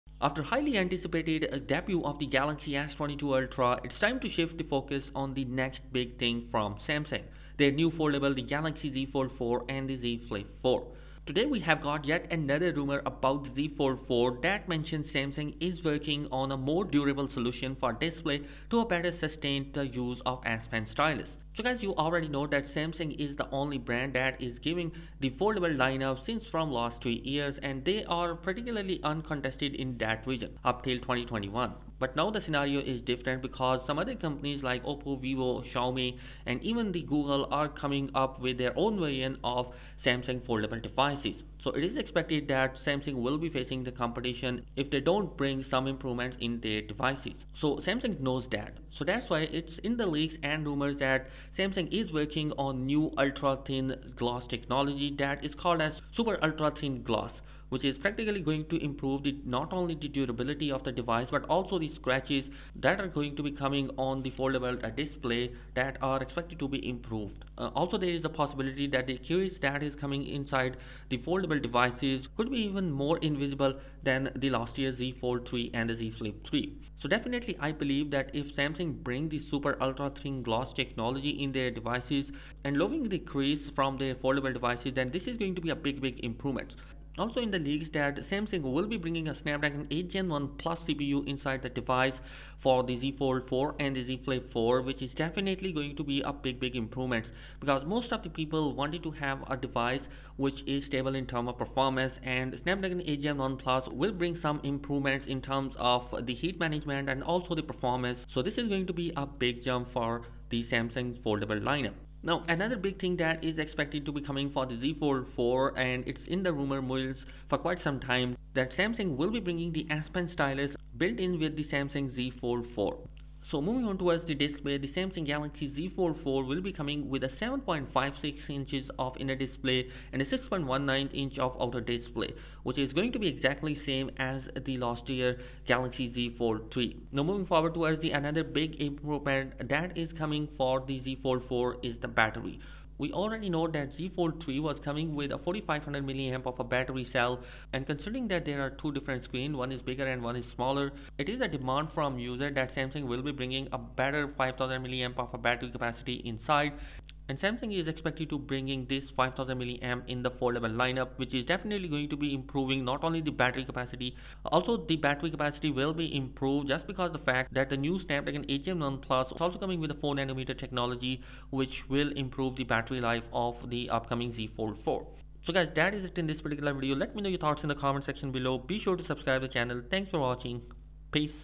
Galaxy Z Fold 4 - Getting Different
Samsung Z Fold 4 has some improvements which make it a device which is getting better in all department expect screen sizes. We have new snapdragon 8 Gen 1 processor, a new Super Ultra Thin Glass for more durability, Spen stylus in the body and expected new camera design.